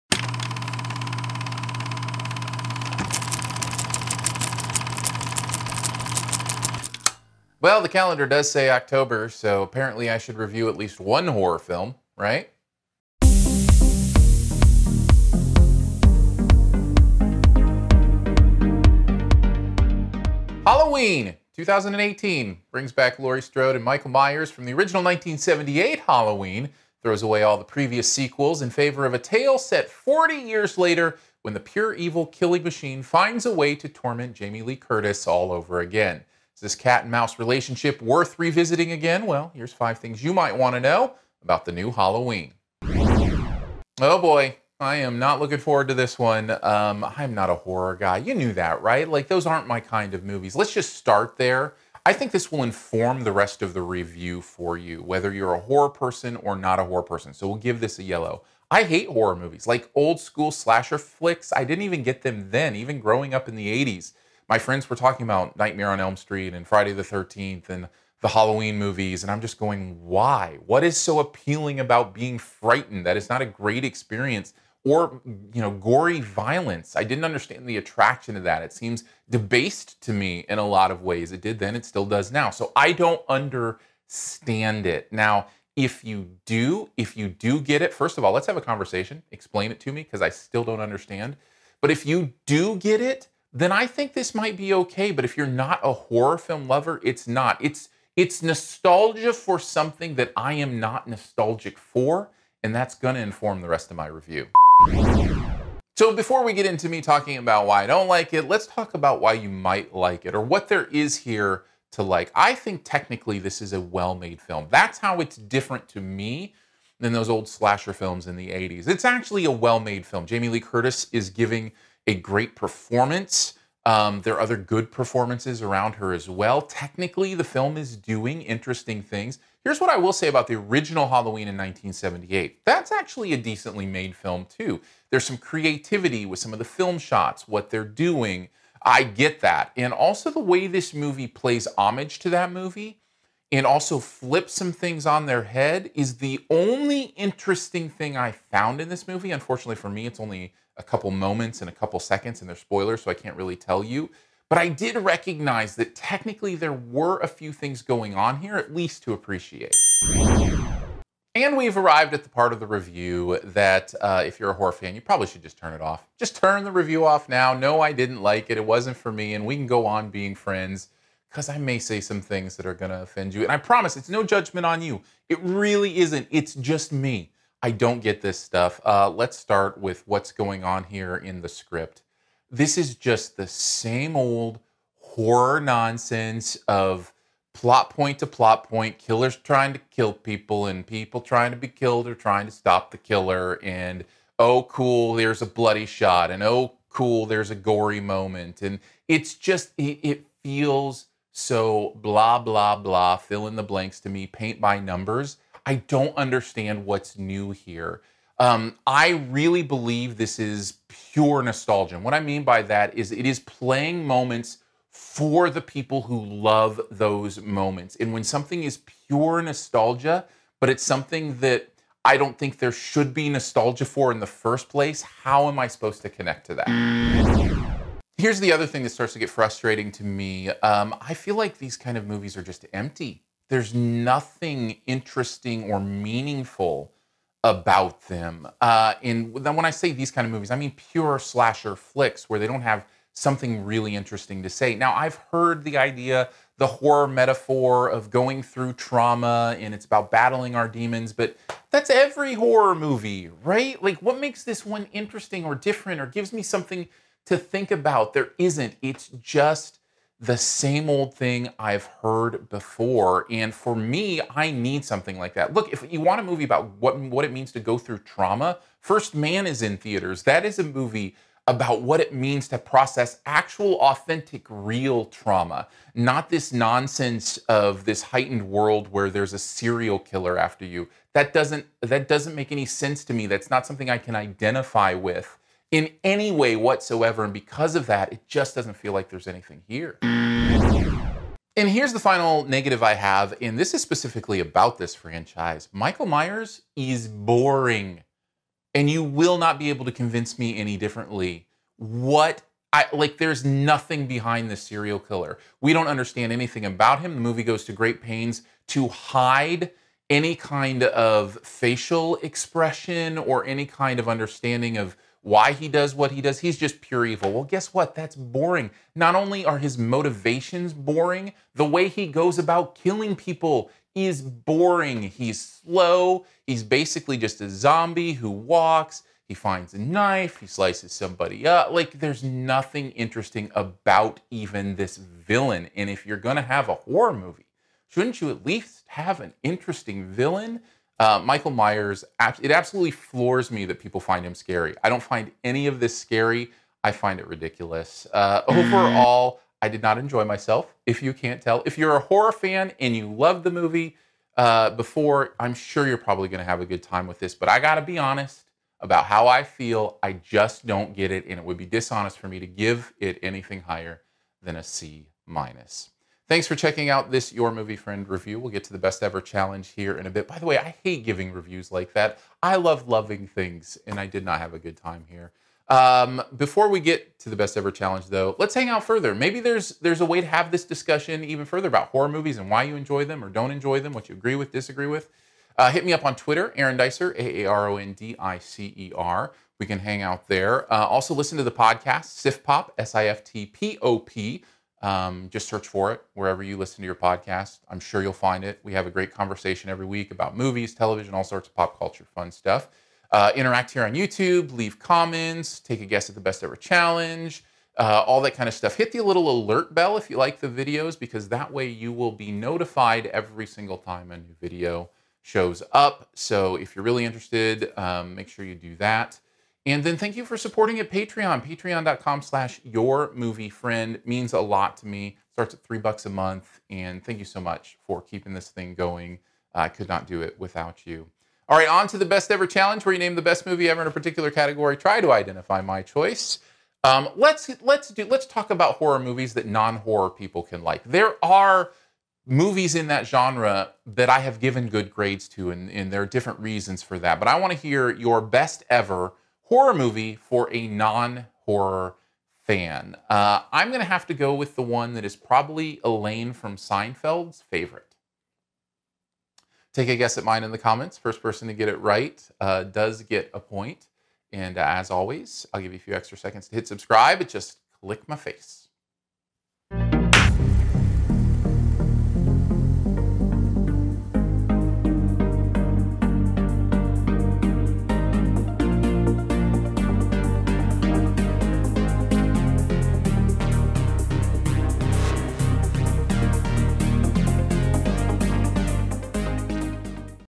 5 Things About "Halloween (2018)" - Movie Review
5 things you might want to know about "Halloween (2018)" spoiler free and in about 5 minutes!

(or search "SiftPop" in your favorite podcast player.)

Follow @aarondicer on Twitter and other social media for more movie conversation.